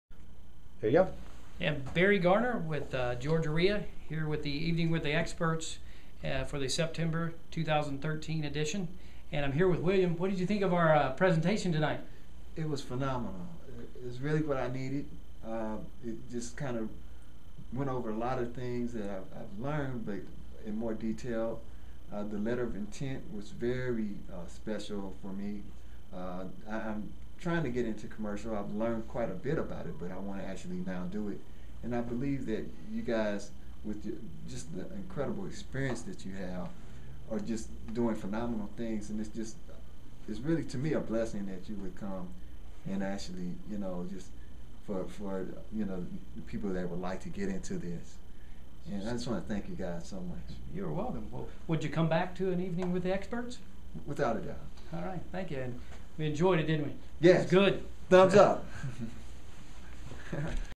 GaREIA Evening with an Expert Training Event on Commercial / Multi-Family Properties
GaREIA Evening with an Expert Training on Commercial / Multi-Family Properties, with Barry Garner, Solutions Realty Network, a GaREIA Business Associate member.